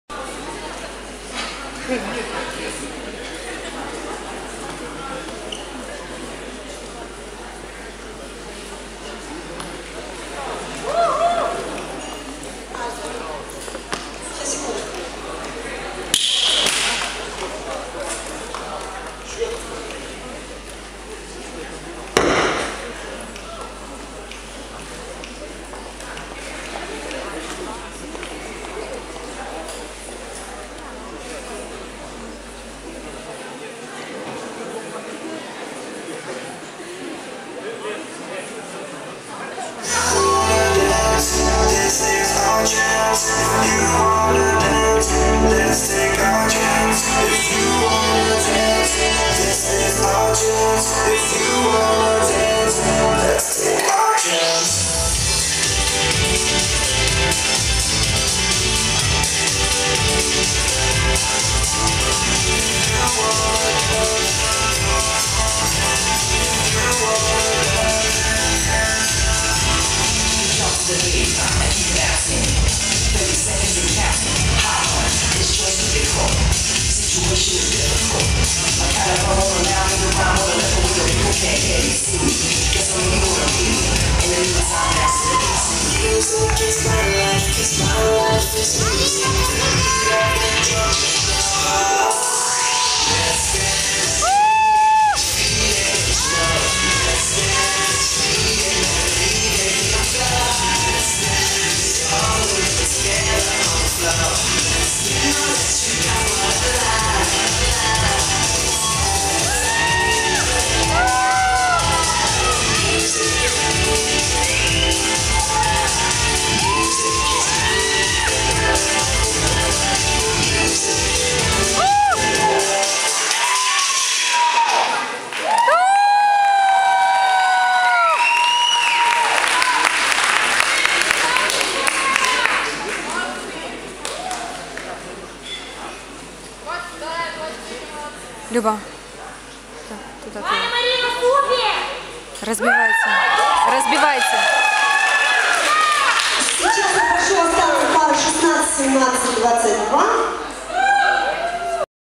Hustle_1.flv
Чемпионат ДВ по хастлу. Абсолют-финал. Быстрая мелодия. общий заход.